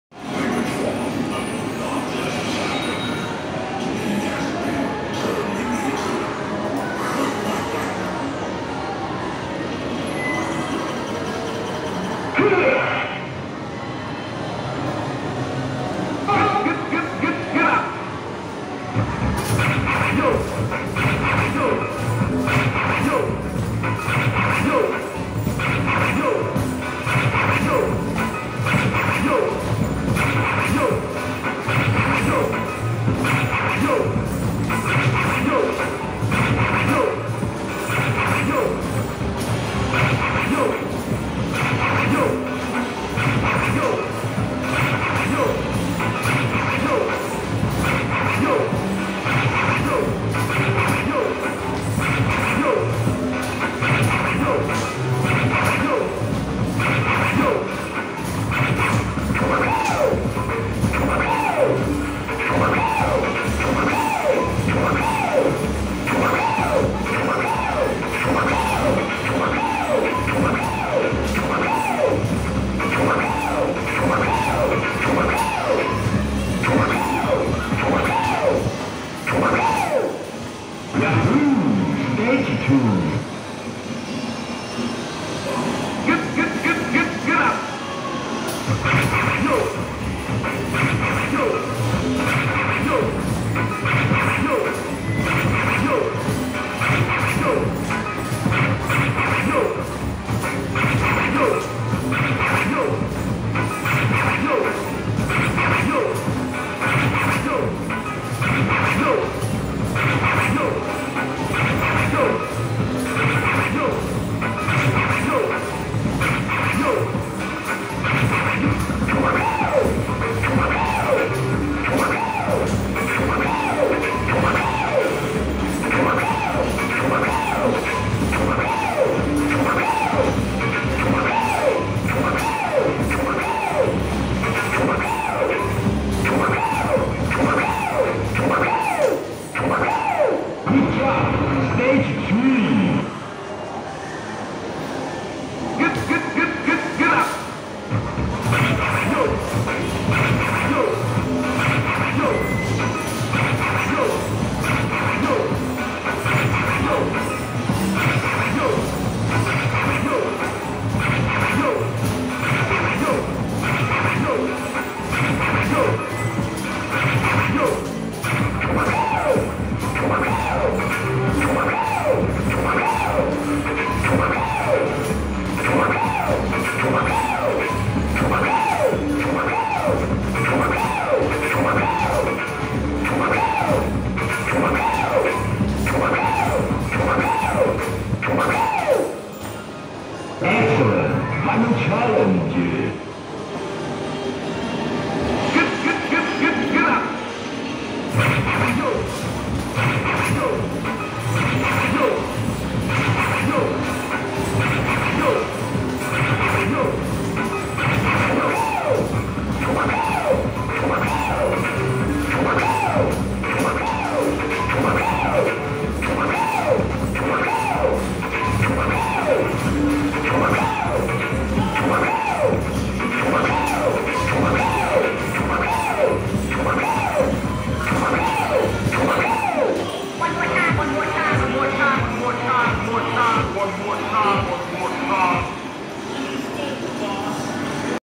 Street Basketball arcade 7 balls 862 scores (2nd and 3rd stage 100% accuracy)
my previous best score in 2021 is 837 scores on last monday. Today I reach 862 scores as my new best score in 2021 so far and this is also first 860+ scores on street basketball practice in 2021 :)

Location : Happy time yogya sumbersari junction Bandung, Indonesia

Street Basketball arcade machine B Ferry William (me)

Stage 1 : 268
Stage 2 : 504
Stage 3 : 704
Stage 4 : 862